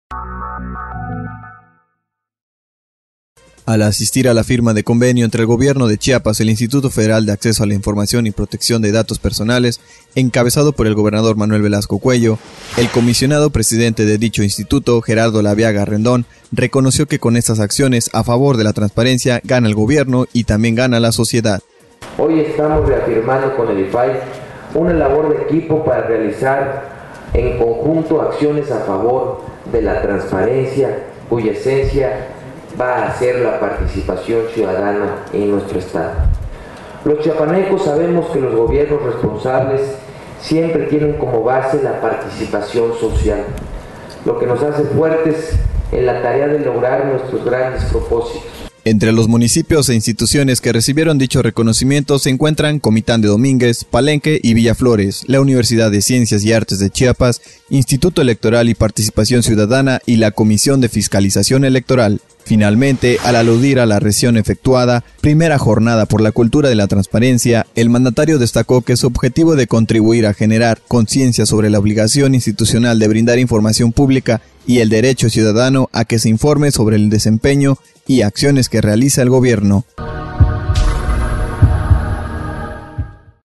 Reconoce IFAI al gobierno de MVC
Al asistir a la firma de convenio entre el Gobierno de Chiapas y el Instituto Federal de Acceso a la Información y Protección de Datos Personales (IFAI), encabezado por el gobernador Manuel Velasco Coello, el comisionado presidente de dicho instituto,

Gerardo Laveaga Rendón, reconoció que con estas acciones a favor de la  transparencia  gana el gobierno y también gana la sociedad.

Laveaga Rendón reconoció al Gobierno de Chiapas por firmar este convenio y apostarle a la transparencia.

Argumentó que una de las grandes prioridades de su gobierno es dar certidumbre a todas las acciones, poniéndolas a la vista y conocimiento de la gente, por lo que invitó a las y los funcionarios chiapanecos a ejercer en todo momento la transparencia en favor de un estado democrático.

Ejecutivo del Estado entregó reconocimientos a municipios y órganos autónomos del estado, que han cumplido en tiempo y forma con las acciones en materia de información pública y buen manejo del Portal de Transparencia.

Entre los municipios e instituciones que recibieron dicho reconocimiento se encuentran: Comitán de Domínguez, Palenque y Villaflores; la Universidad de Ciencias y Artes de Chiapas;  Instituto Electoral y Participación Ciudadana y la Comisión de Fiscalización Electoral.

Finalmente, al aludir a la recién efectuada "Primera Jornada por la Cultura de la Transparencia", el mandatario destacó que su objetivo es contribuir a generar conciencia sobre la obligación institucional de brindar información pública y el derecho ciudadano a que se informe sobre el desempeño y acciones que realiza el gobierno.